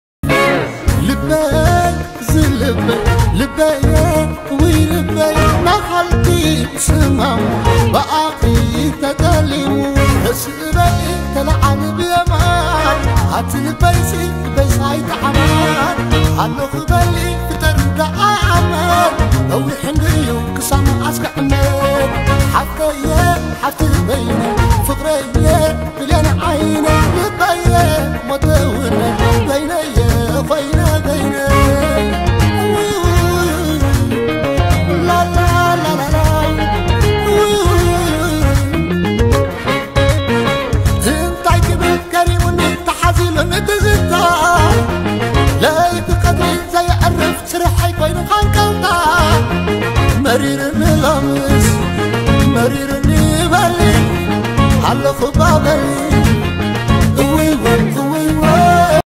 seare weldemichael
ን ዘሎን ዝነበረን ዝነብርን ፍቕራዊት ቃንዛ እትገልጽ